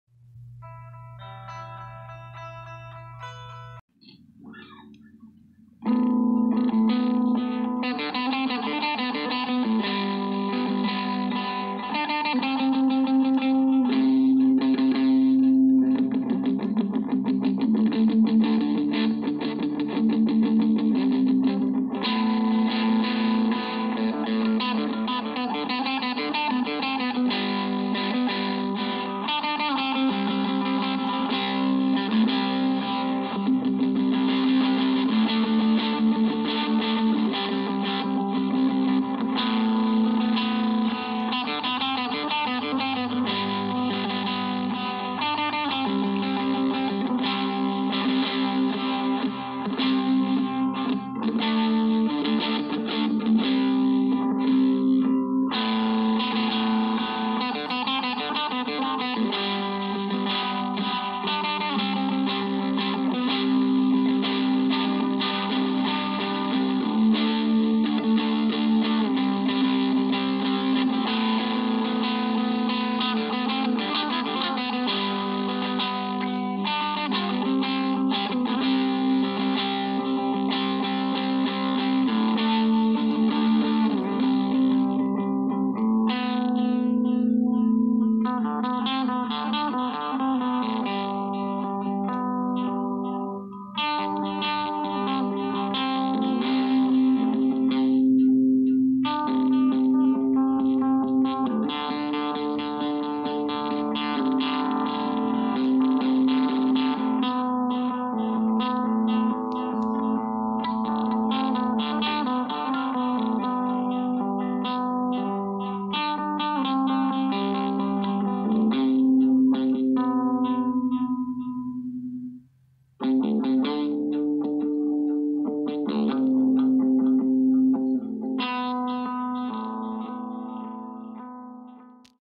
More RARE music from the Guitar VAULT
Another unreleased track I came up with on my 7 string guitar.

Intro music and all additional guitar tracks by Kris Stazel